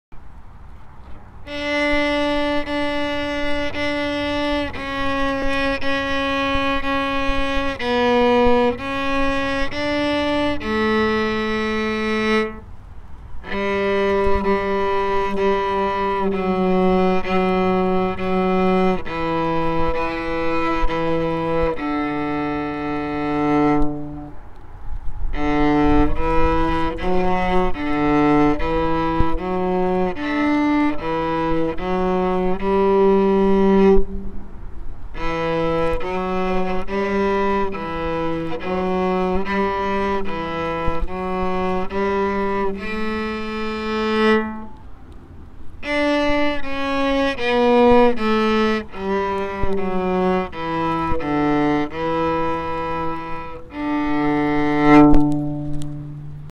French Folk Song
French Folk Song. The tunes are slow and soulful. It is played moderato on A & D strings. It has a long singing sound.